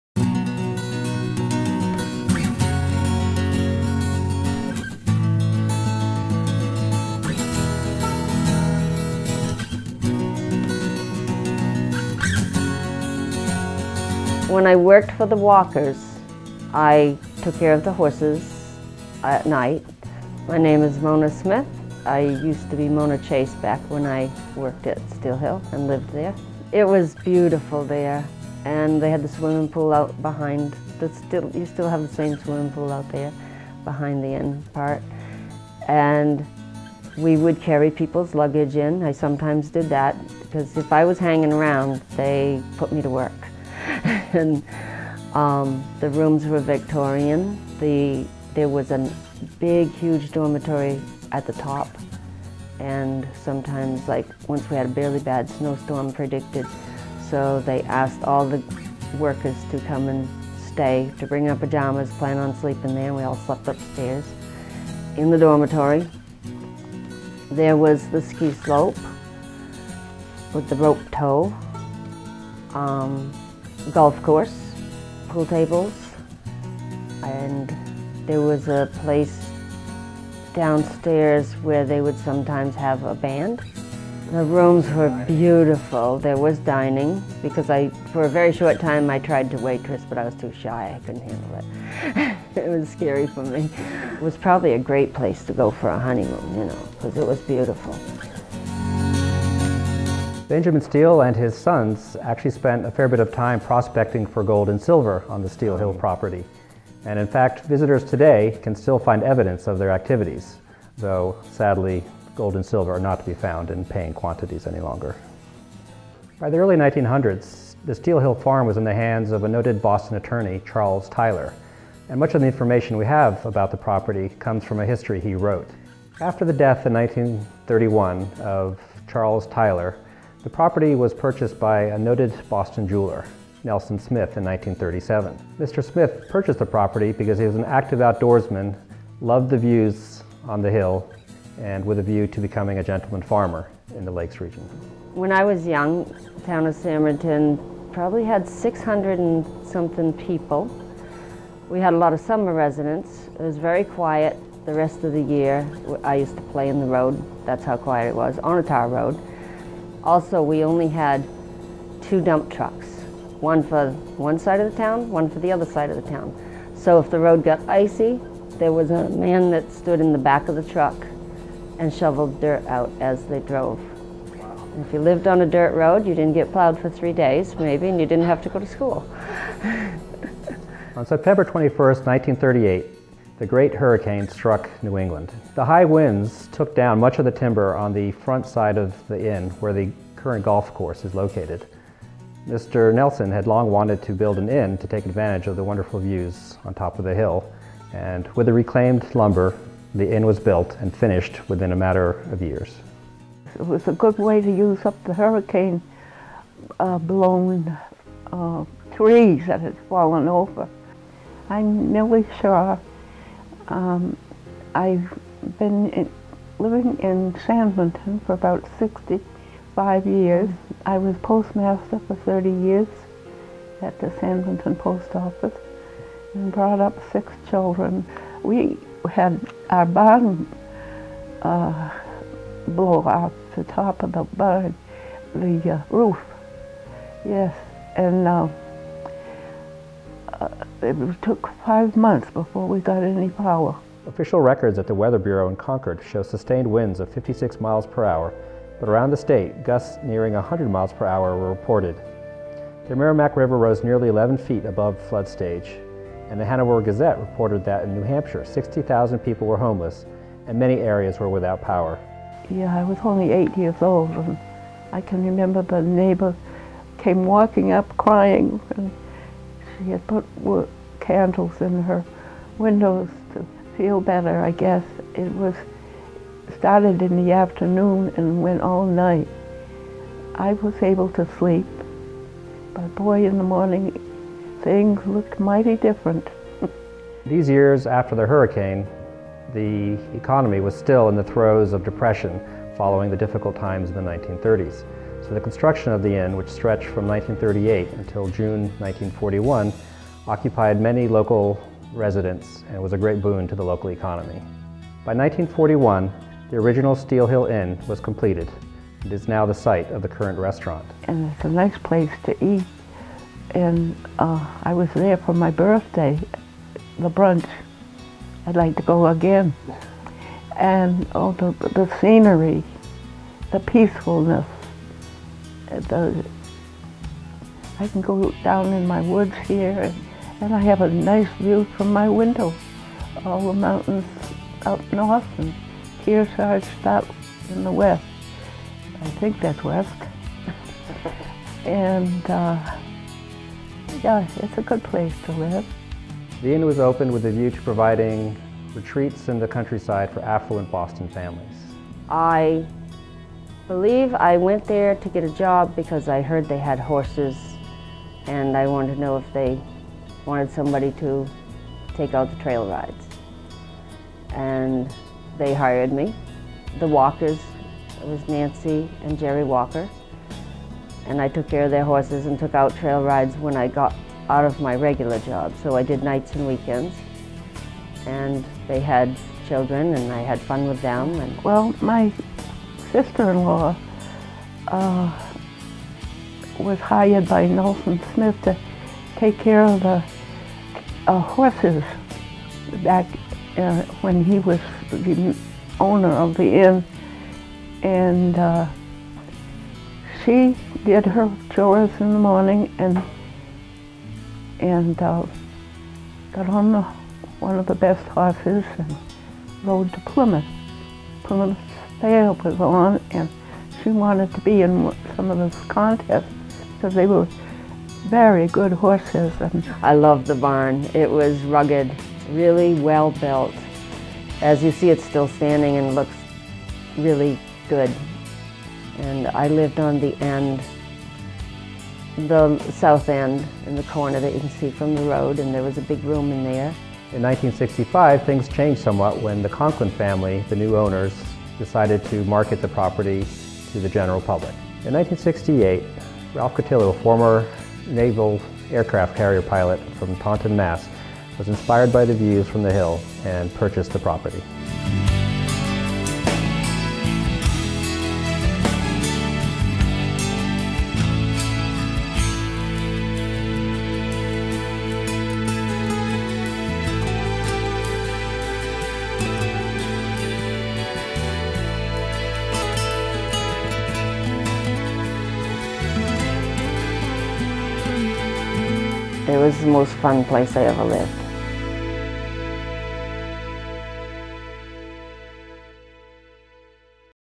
Hurricanes, Horses and Cows - a history of Steele Hill Resorts
A documentary examining the early history of Steele Hill Resorts.  Footage includes first hand accounts of the early years before the main inn was built and historical facts about the hurricane of 38 felling the timber that was later used to construct the original building.